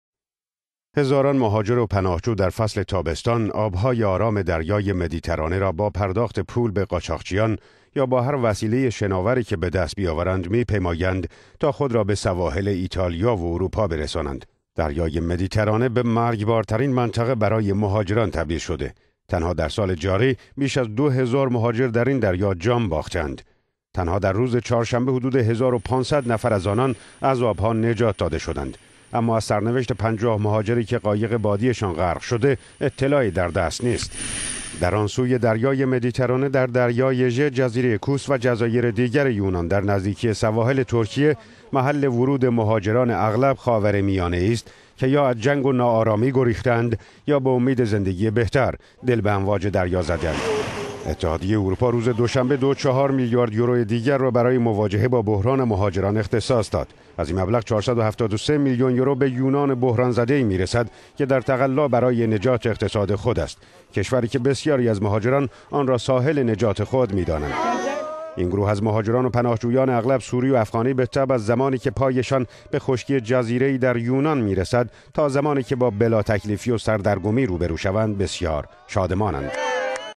بحران در دریای مدیترانه؛۵۰ مهاجر دیگر در آب ها ناپدید شدند
هزاران مهاجر و پناهجو در فصل تابستان، آب های آرام دریای مدیترانه را با پرداخت پول به قاچاقچیان و یا با هر وسیله شناوری که بدست بیاورند می پیمایند تا خود را به سواحل ایتالیا و اروپا برسانند.

تنها در روز چهارشنبه حدود ۱۵۰۰ نفر از آنان از آب ها نجات داد شدند اما از سرنوشت ۵۰ مهاجری که قایق بادی شان غرق شده اطلاعی در دست نیست.

دریای مدیترانه به رغم امواج آرامش در تابستان گرم امسال به مرگبارترین منطقه برای مهاجران تبدیل شده است. تنها در سال جاری بیش از ۲۰۰۰ مهاجر در این دریا جان باخته اند.